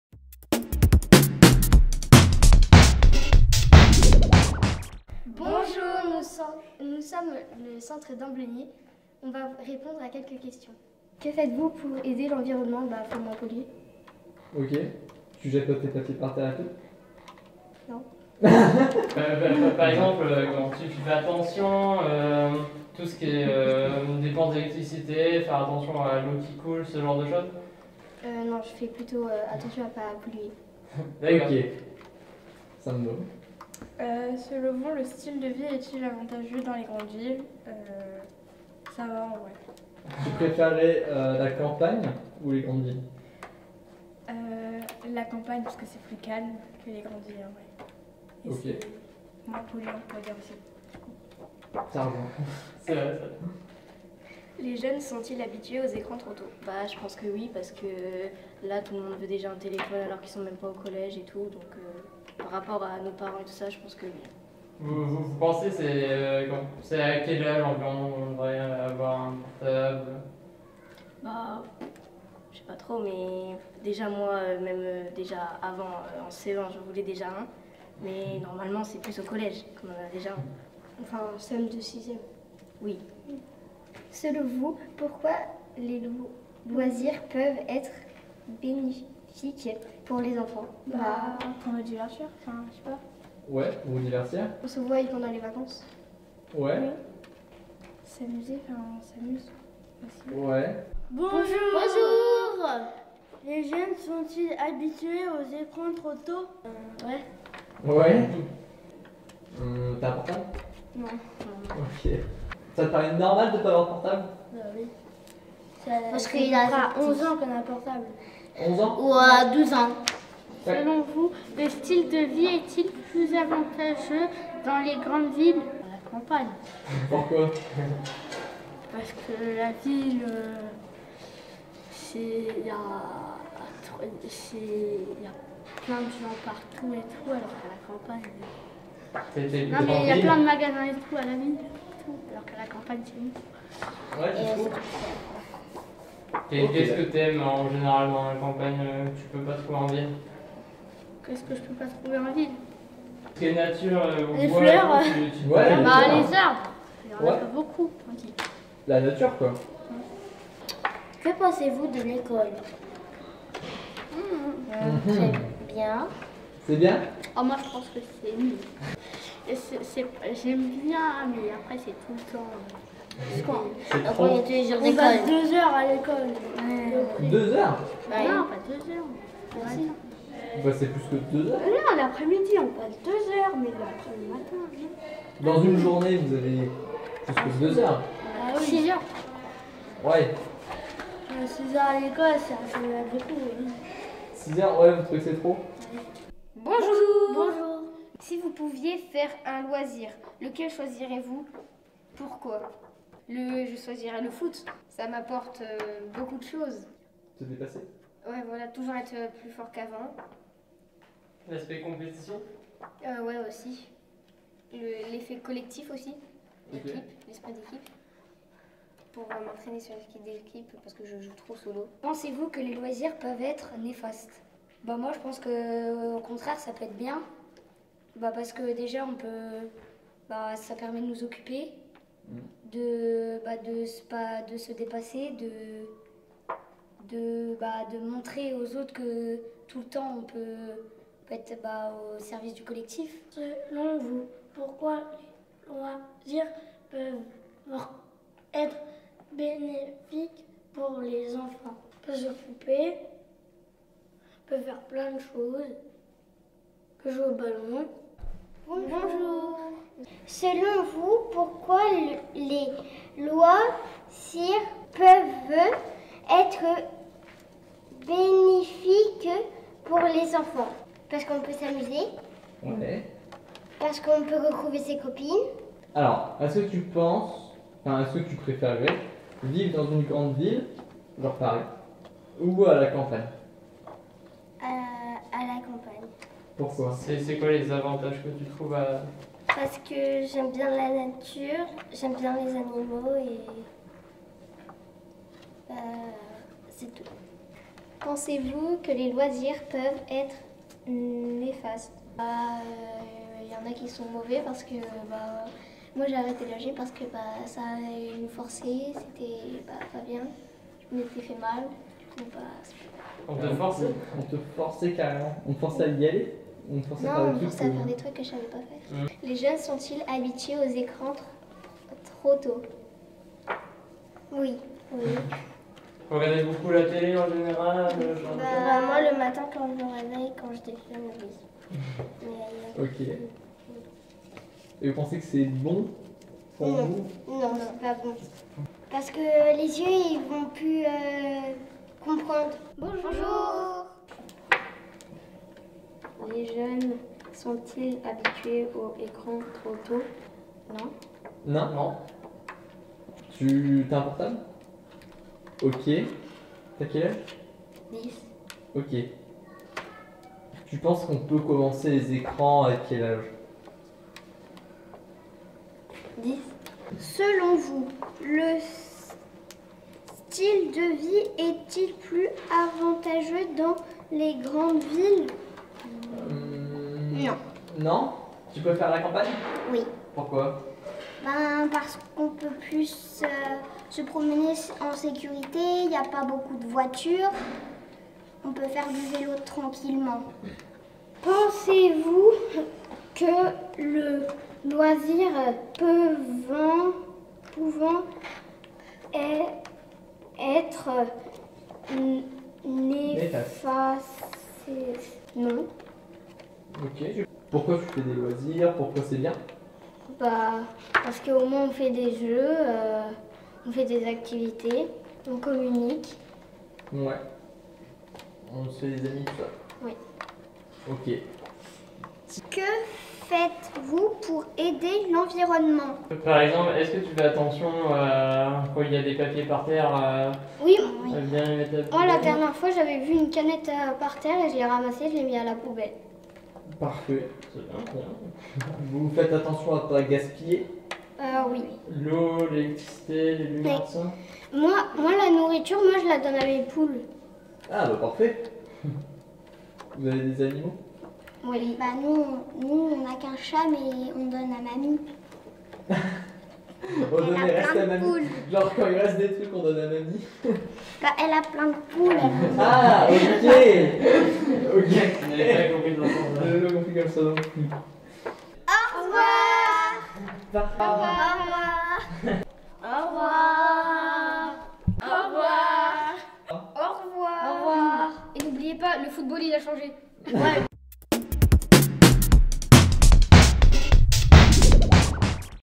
Le P'tit Média - Interview des enfant du centre de loisirs d'Ambleny
Le P'tit Média prend ses quartiers d'été!
Lors des centres de loisirs, le P'tit Média s'est rendu à Ambleny afin de donner la parole aux enfants. Ils ont eu l'occasion de s'exprimer devant la caméra pour répondre à des questions sur des sujets de société;